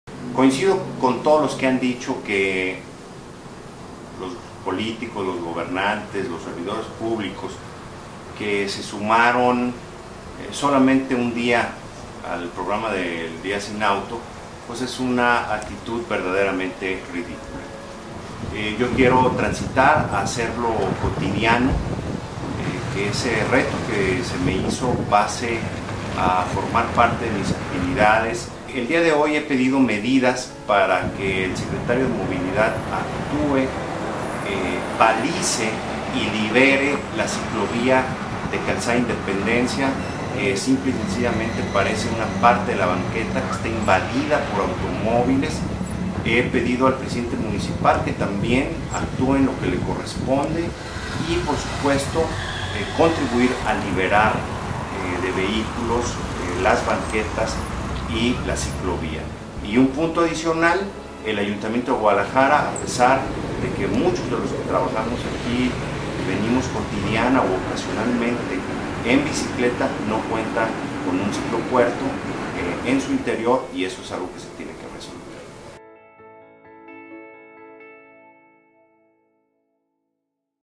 Al trabajo en bici.
Agradezco a Carlos López Zaragoza haber despertado en mí la conciencia de utilizar medios alternativos e ir disminuyendo el uso de mi automóvil.

Decidí continuar realizando mis trayectos laborales en bici, al menos un día a la semana. Les voy a compartir las irregularidades con las que me encuentro en mis recorridos como peatón, ciclista y usuario del transporte público.

Además, hago un llamado al Secretario de Movilidad para que libere y balice la ciclovía de la Calzada Independencia en favor de los ciudadanos.